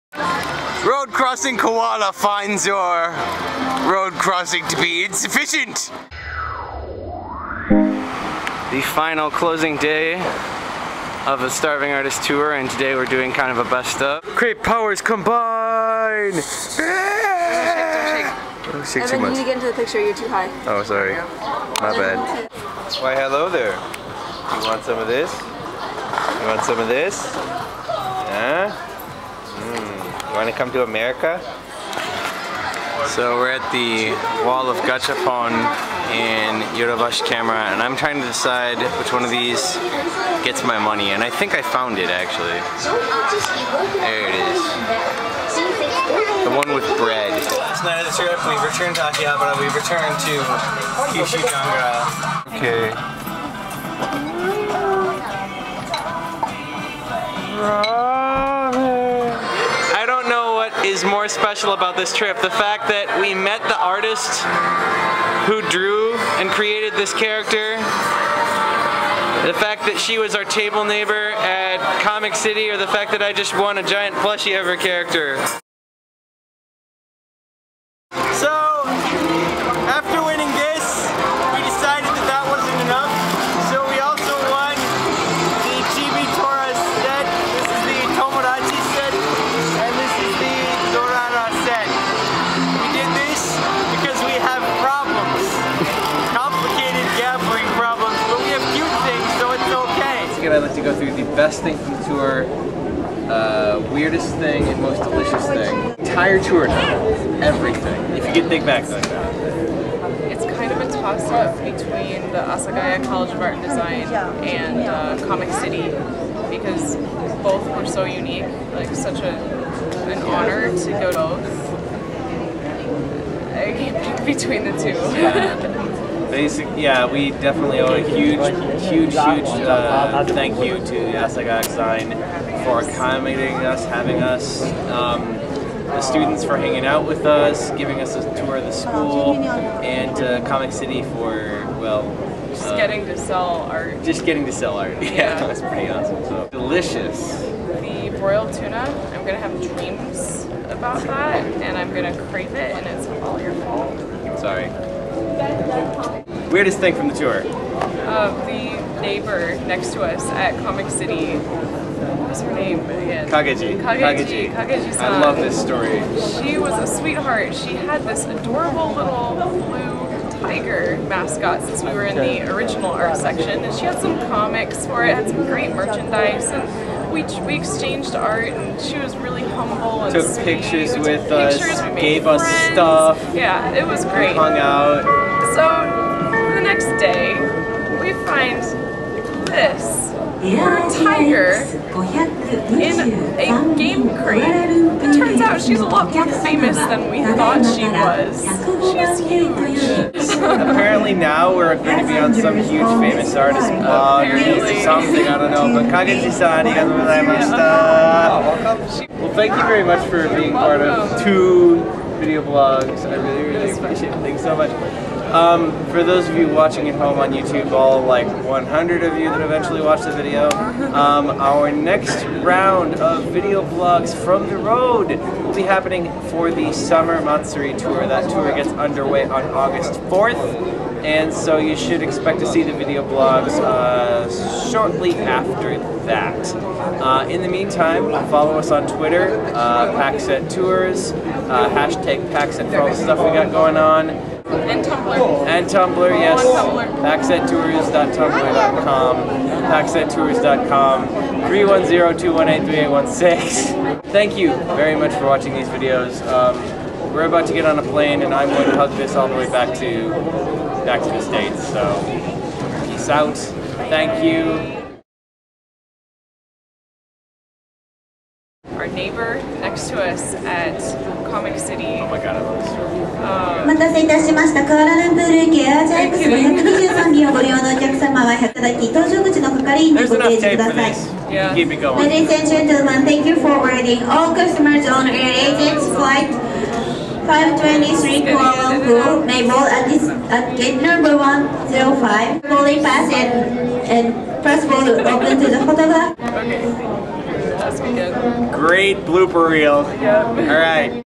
Starving Artist 2012 Vlog 6: Tour Wrap-up
***SIGN UP FOR THIS TOUR FOR 2013!

The Starving Artist Tour concludes with our sixth and final vlog from the road! On tap: more Harajuku, more Akihabara, and the awesome story of how we befriended one of Japan's hottest up-and-coming Character Designers. Also: a few bloopers.